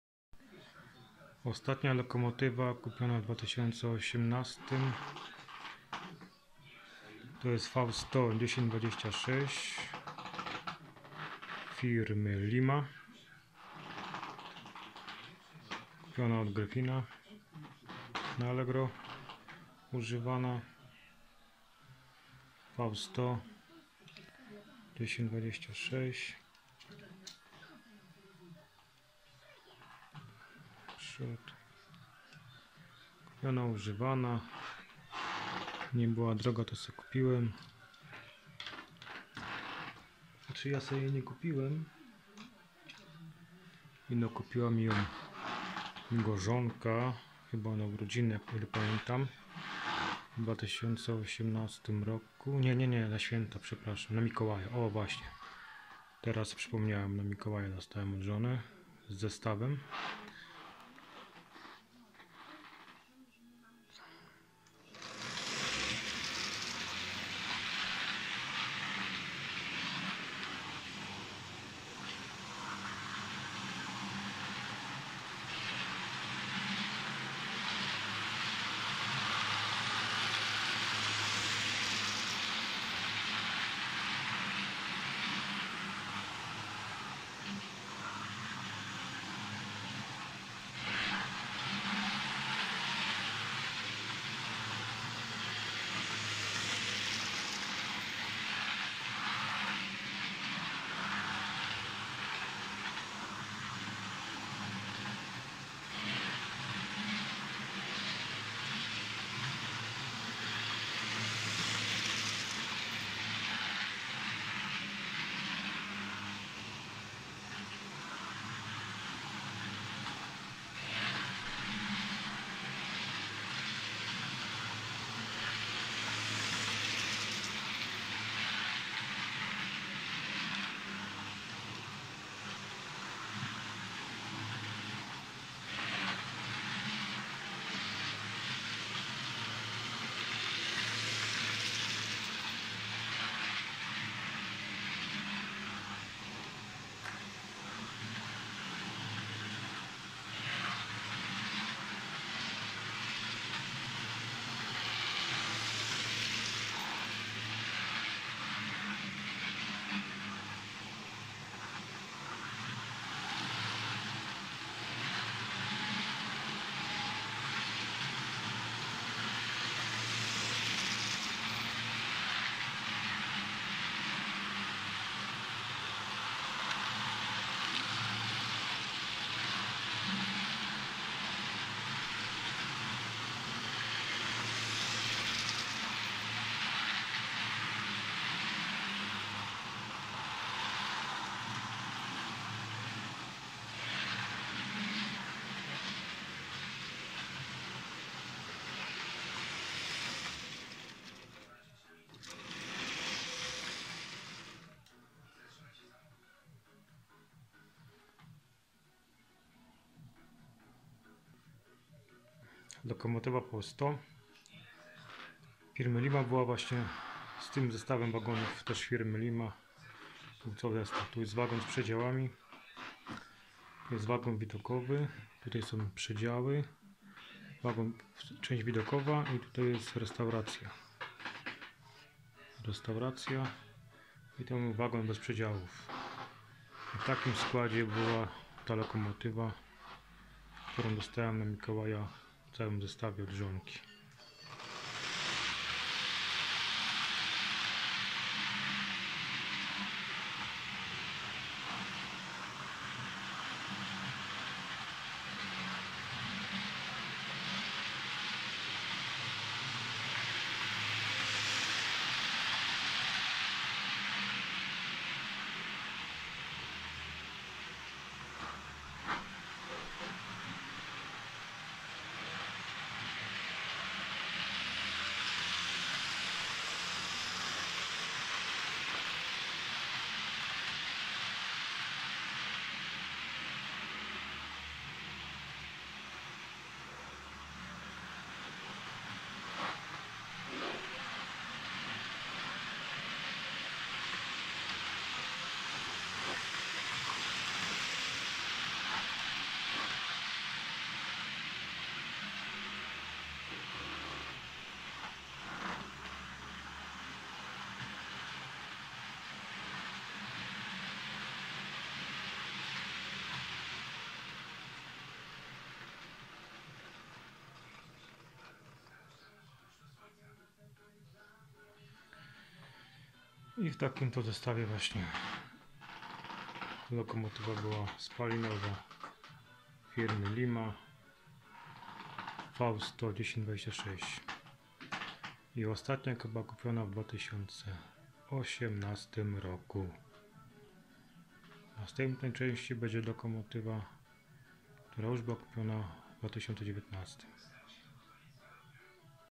12/2018 Spalinowóz V 100 1026 Lokomotywa firmy LIMA 1:87 H0
Ostatnia lokomotywa kupiona w 2018 roku model  firmy LIMA, dostałem od żonki na Mikołaja z trzema wagonami osobowymi cały zestaw był kupiony używany

Lokomotywa :Spalinowóz V 100 1026